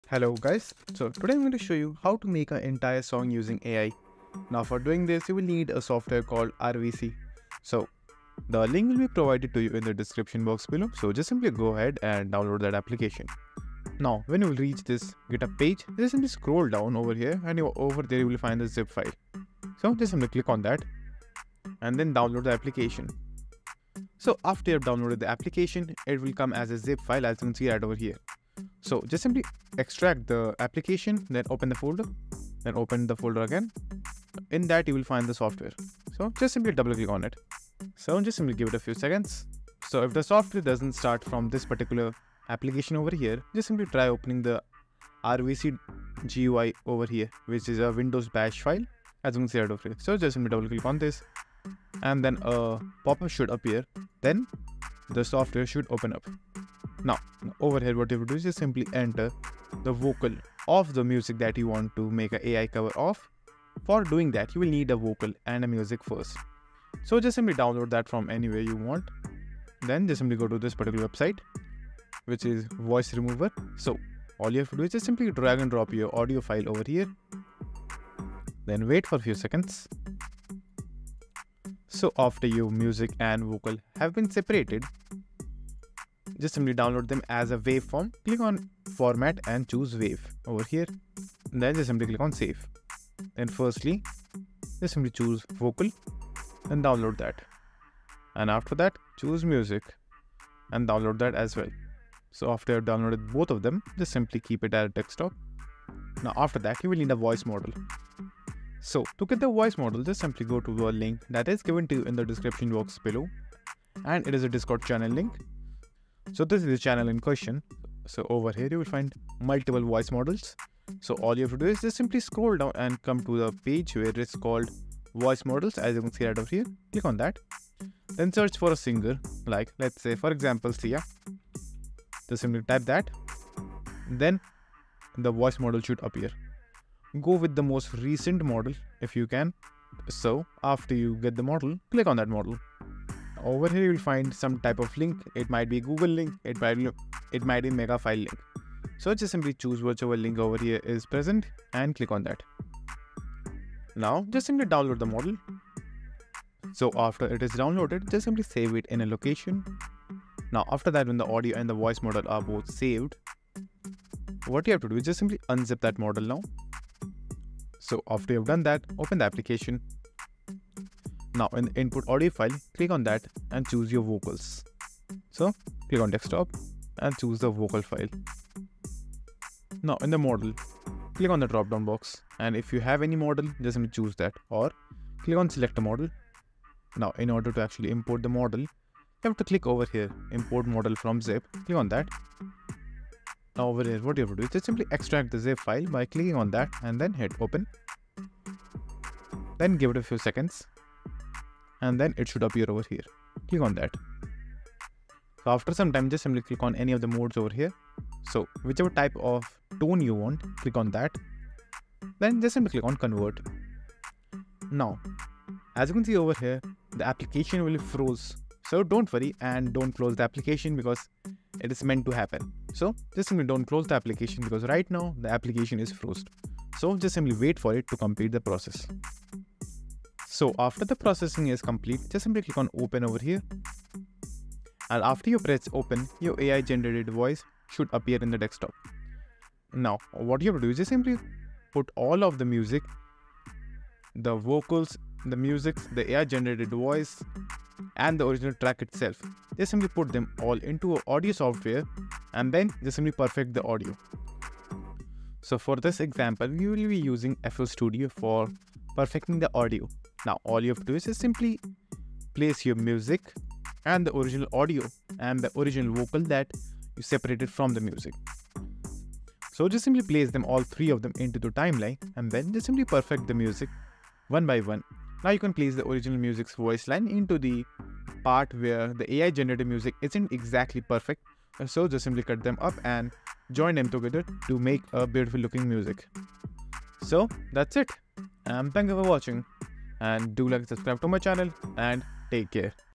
How to Make an Entire Song & Covers Only Using AI | RVC Tutorial (Free AI Music Generator 2023)
How to Make an Entire Song Only  Using AI  ( Free AI Music Generator 2023)

Create an entire song using only AI using this free AI music generator: RVC Software

With this simple and exciting process, you can have a unique song.

Just choose the style, tempo, and mood, and let the AI compose the music for you.

No musical expertise is needed!